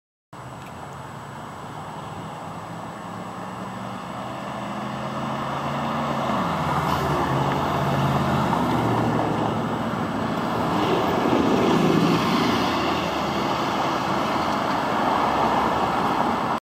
Burlington Transit bus 7058-12 crossing back into Burlington, Ontario
route: 1 Plains